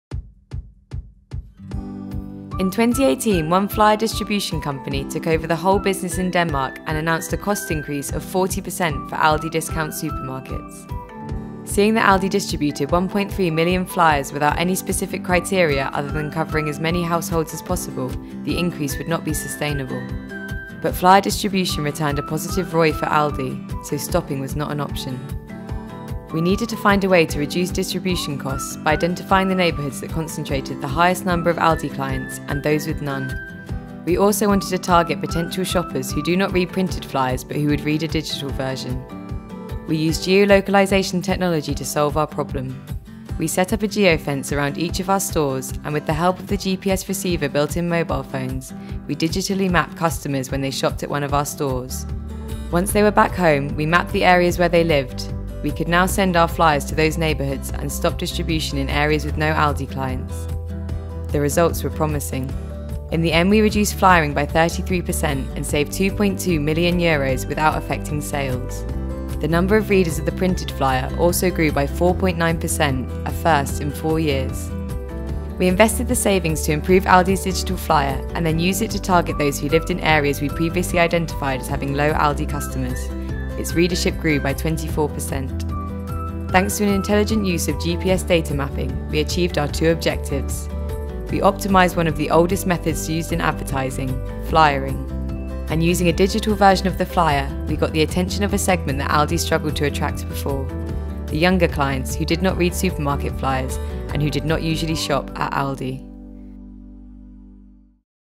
ALDI - Creating Retail Traffic Through Intelligent GPS Data Customer Mapping
Using mobile geolocalization technology, ALDI avoided an expected 40% increase in flyer distribution costs by identifying the neighborhoods that had the highest concentration of ALDI clients and those with few or none. We reduced distribution in low interest areas saving €2.2M and used the savings to invest in improving ALDI’s digital presence to attract a much more modern segment.